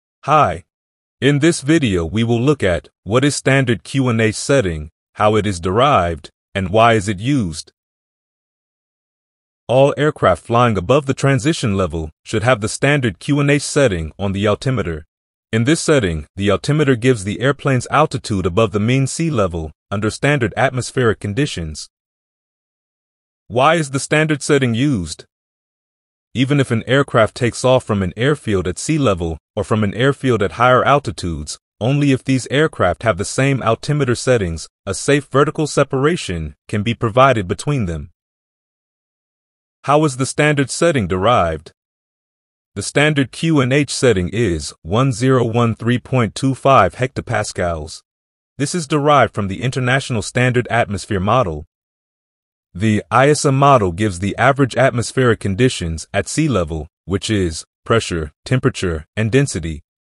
What is STANDARD QNH?? | Why is it used? | How it is derived?
All aircraft flying above the transition level should have Standard QNH setting on the altimeter for maintaining a safe vertical separation from other aircraft. In this video we see what is this setting, why it is used and how it is derived.